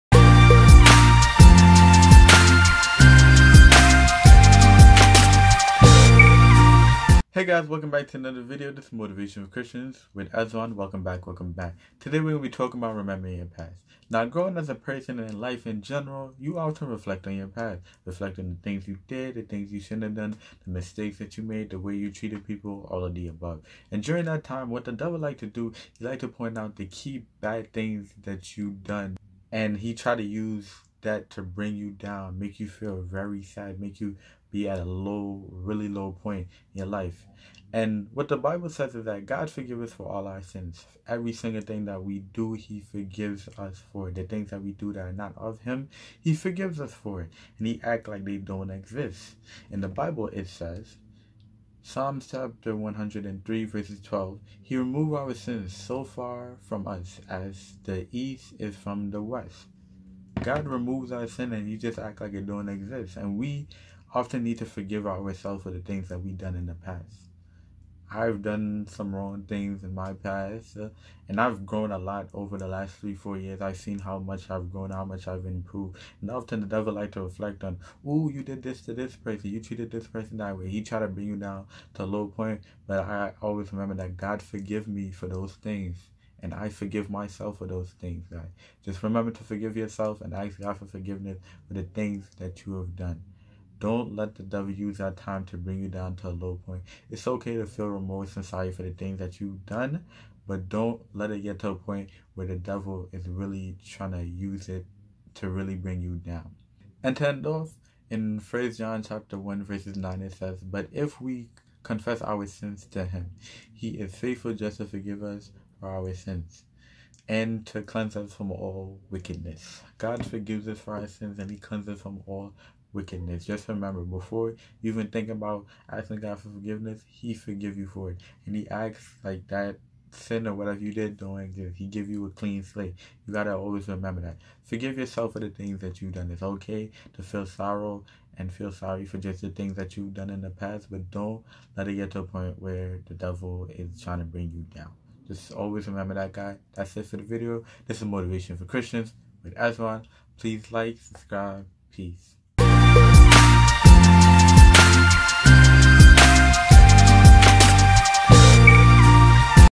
Remembering Your Past |MFYC
In today's video, I talk about remembering your past.

-Executive Producer: Esron Holder And Jave
-Videographer: Esron Holder
-Editor: Esron Holder